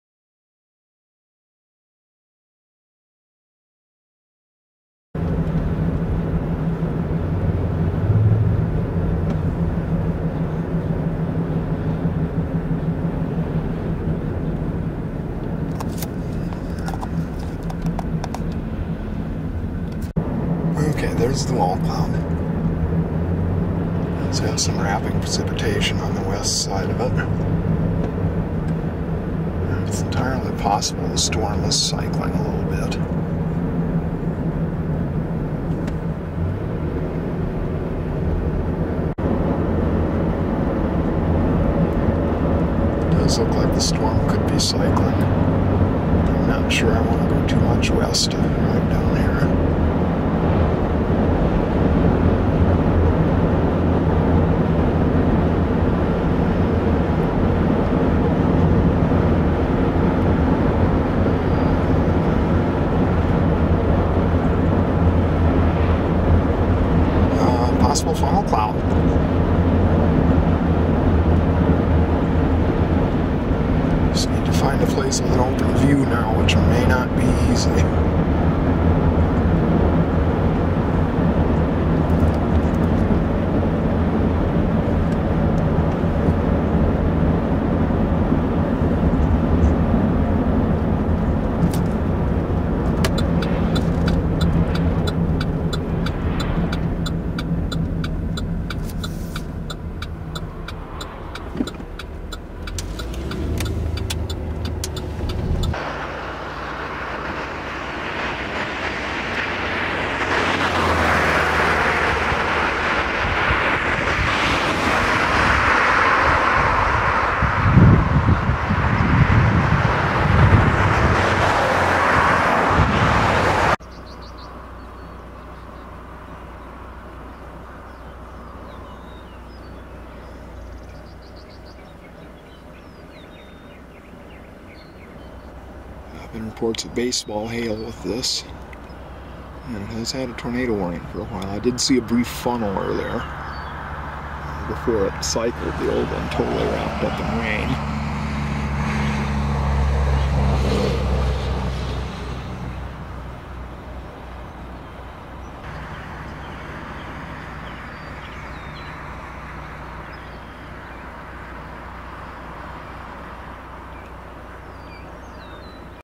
Supercell and Funnel Cloud, W. of Bolivar, MO, 5/12/11
View of tornado-warned supercell and a distant but nicely backlit funnel cloud from the Bolivar, MO area.  Video starts with view of updraft tower from downtown Bolivar around 5:40 p.m., then a view of the funnel cloud while driving just west of Bolivar around 5:45.  The funnel was likely over or coming off Stockton Lake.  By the time I could find a place to stop with a clear view, the funnel was gone and the wall cloud began to wrap in rain.  A new, less well-organized meso then formed northeast of the earlier one that had produced the funnel.